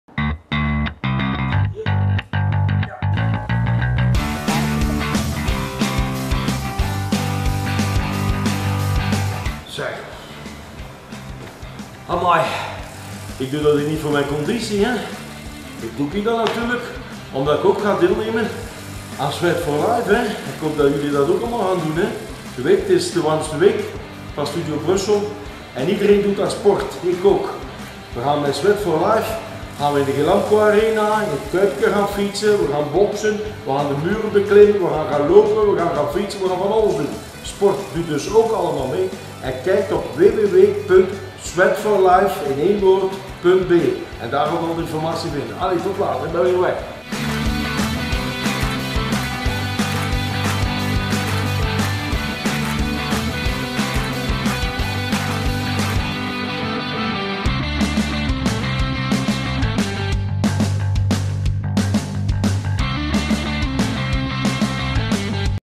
Sweat For Life - Daniël Termont
Tijdens Sweat for Life willen we ons, samen met een hoop sportieve Gentenaars, in het zweet werken ten voordele van het goede doel. Hierbij staat een gezellige, sportieve sfeer centraal en willen we vooral een hoge funfactor bieden: booty-workouts, in groep fietsen op rollen, bokslessen, een nachtloop, workouts in het water, klimmen, squatsessies, enz. allen in het teken van de Warmste Week van Studio Brussel.